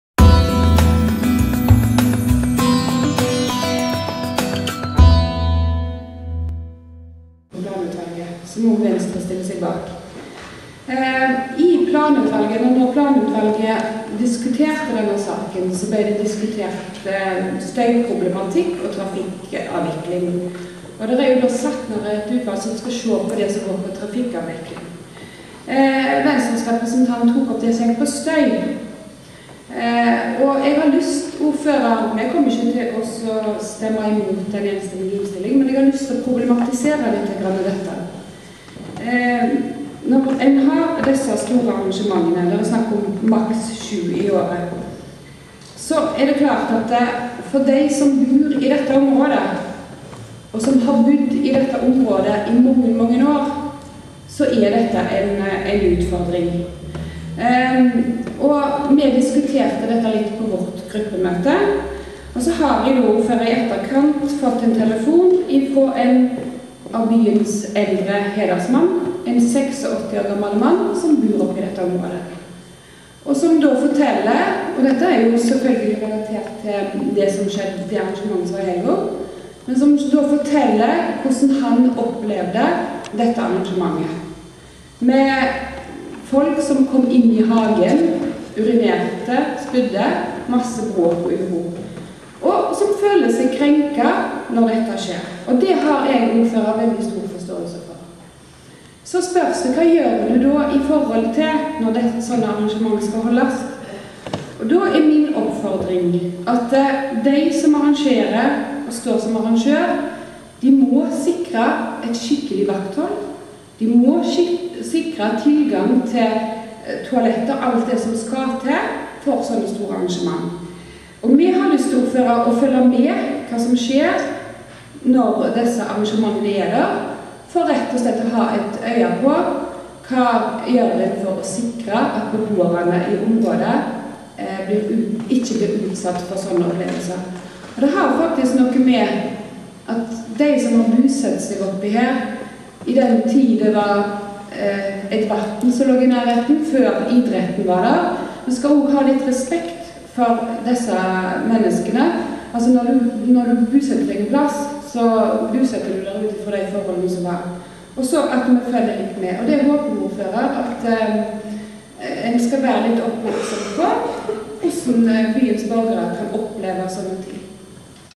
Om regulering av Vardhallen
May Britt Vihovdes innlegg i bystyret om omregulering av Vardhallen